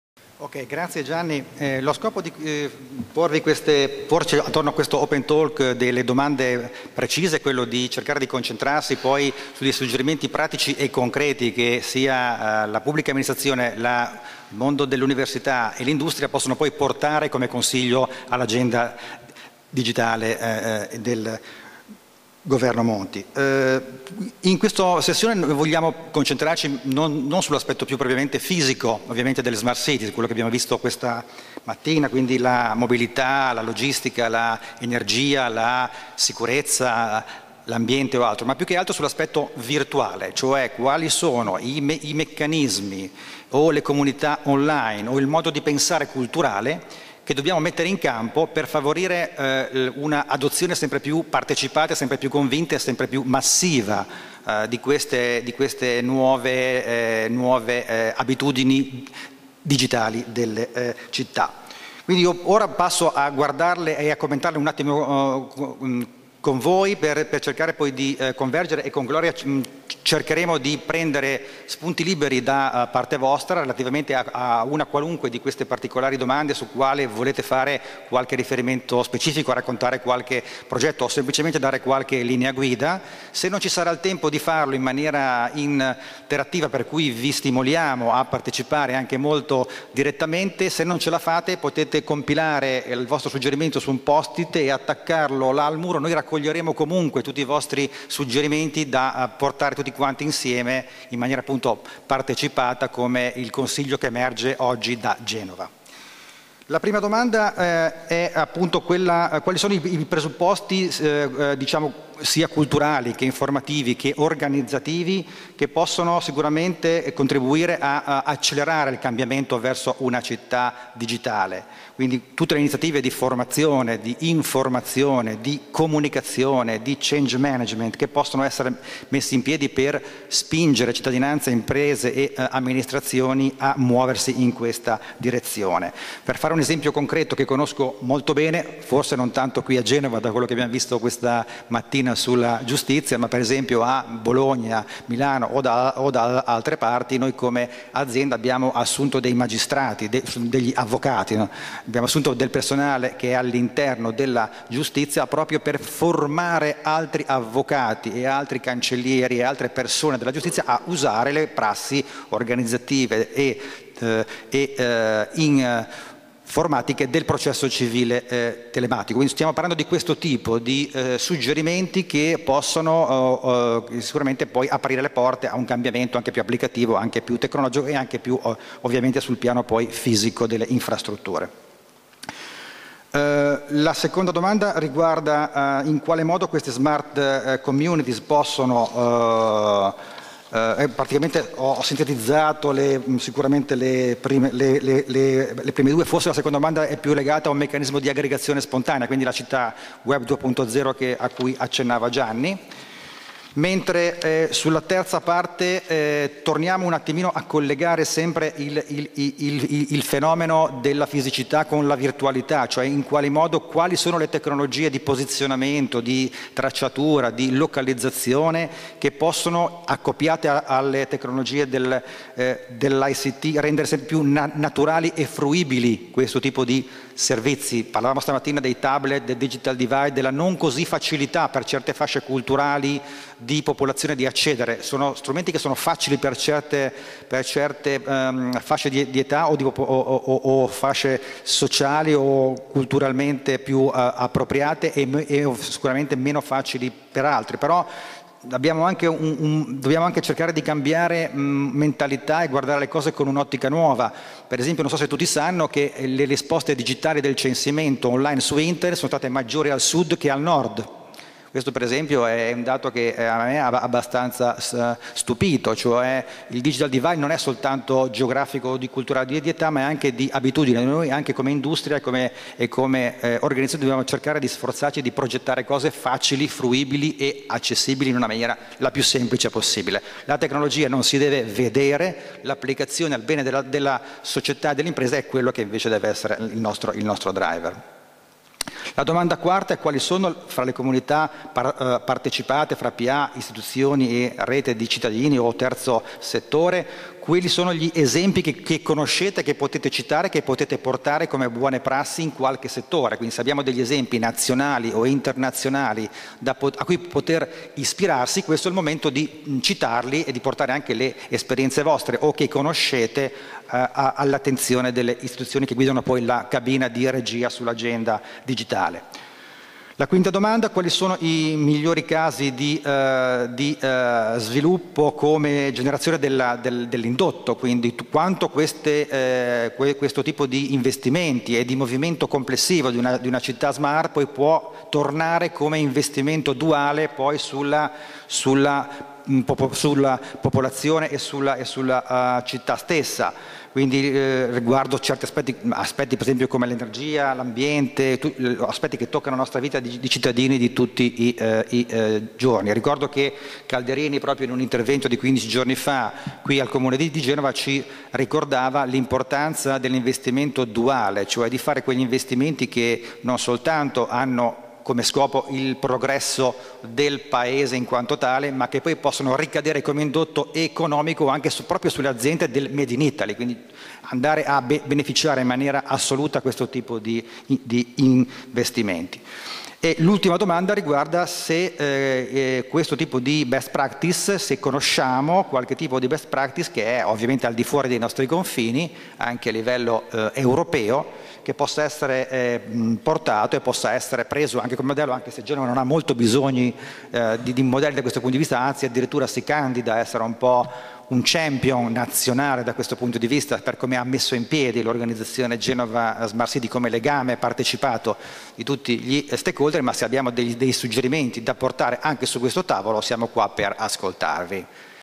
22 Danilo Bianco
Coordinatore/Shaker Danilo Bianco (Responsabile Marketing SELEX Elsag).
Convegno "Genova per l'Innovazione Digitale, an Open Smart City" del 23 marzo 2012.